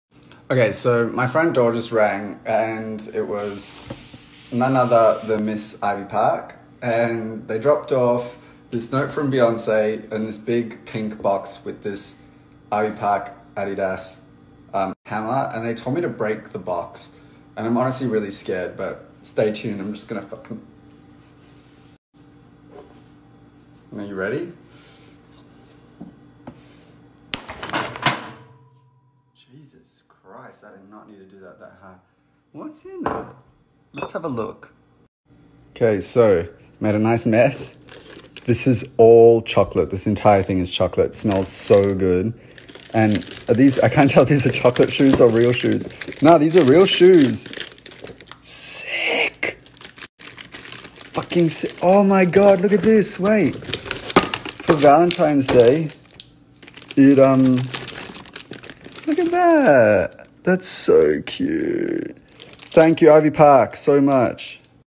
Troye opening Ivy Park x Adidas PR Package
Troye shares to his Instagram Story him opening a PR Package he received from Ivy Park’s Valentines Collection